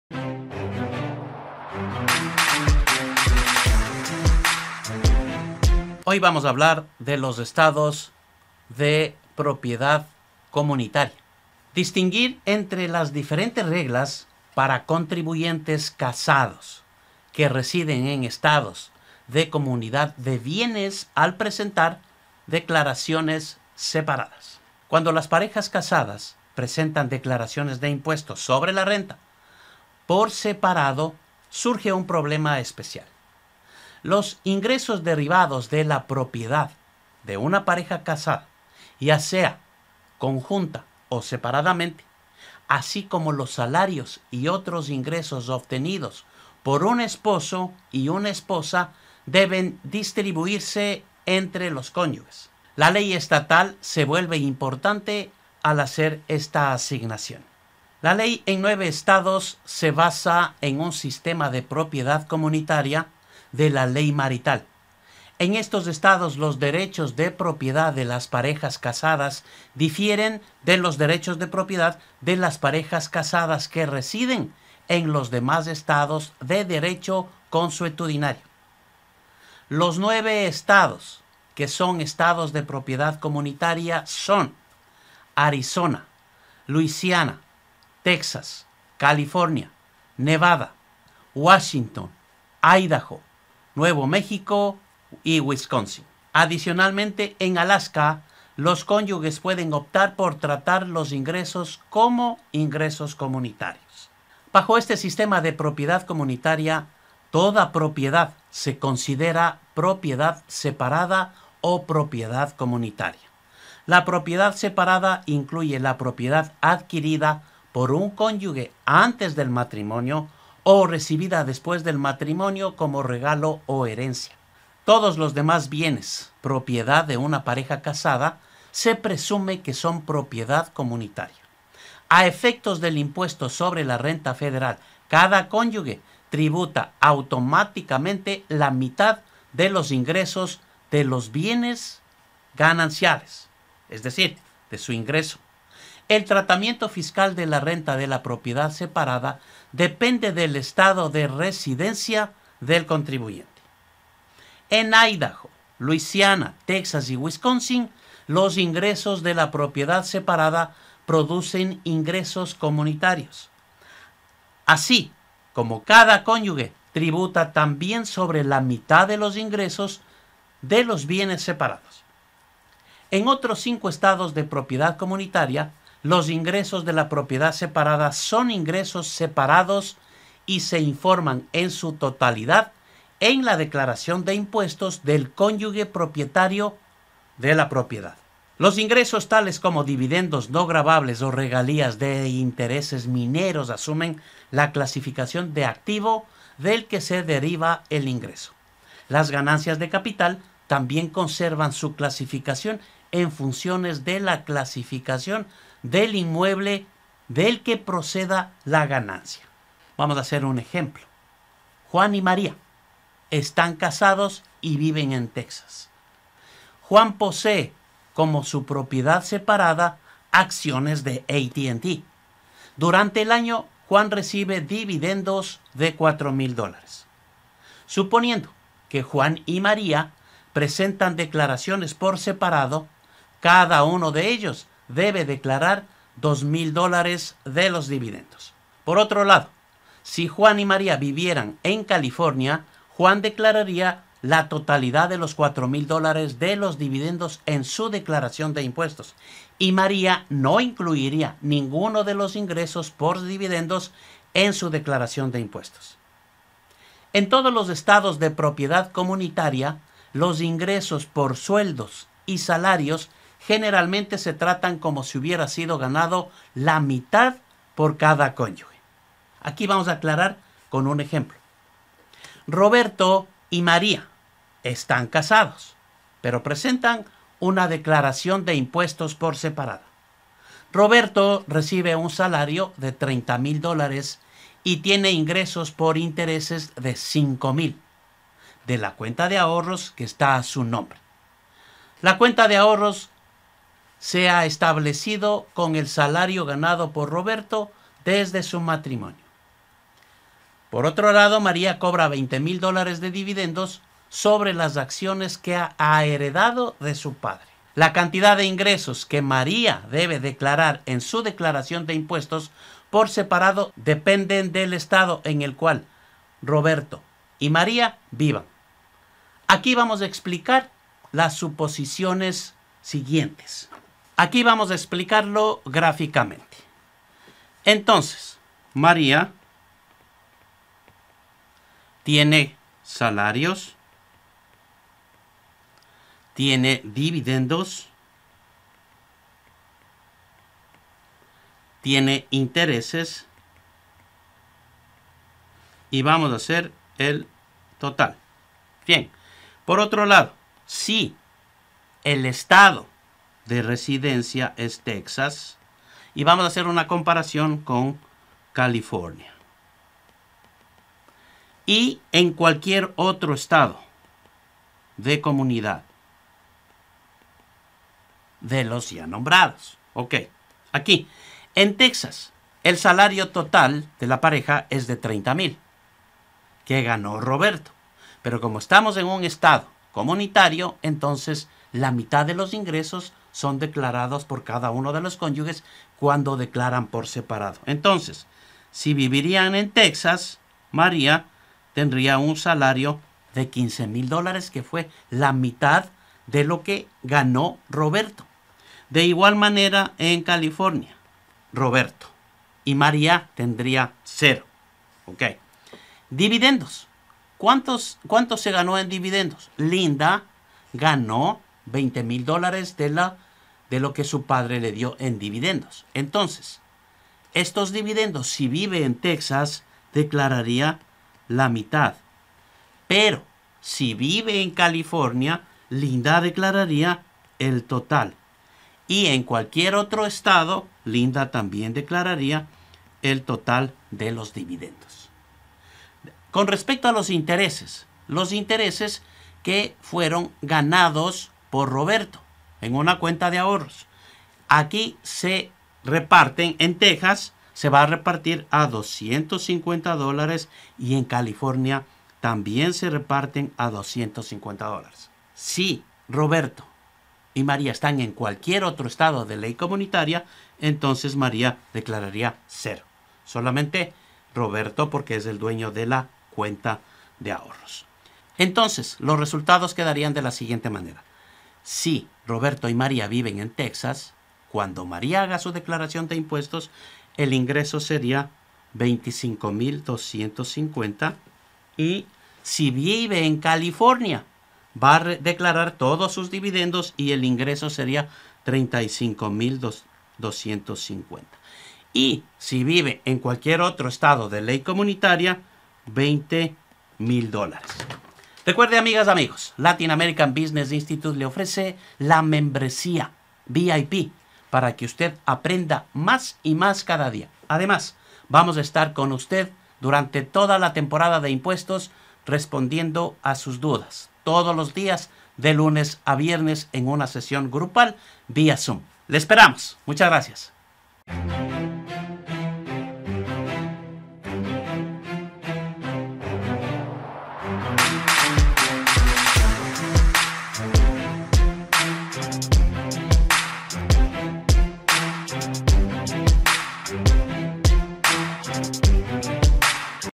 Estados de Propiedad Comunitaria - Cápsulas Tributarias - Impuestos en Estados Unidos
𝗖𝗔𝗣𝗦𝗨𝗟𝗔𝗦 𝗧𝗥𝗜𝗕𝗨𝗧𝗔𝗥𝗜𝗔𝗦 con Carlos Ramírez EA, MBA, NTPI Fellow
Hoy nos explica ESTADOS DE PROPIEDAD COMUNITARIA.
Aprenda este y otros temas tributarios en Labitax.

MI PÁGINA OFICIAL - CARLOS RAMIREZ EN FACEBOOK
SÍGAME EN FACEBOOK Y ENTÉRESE DE TODO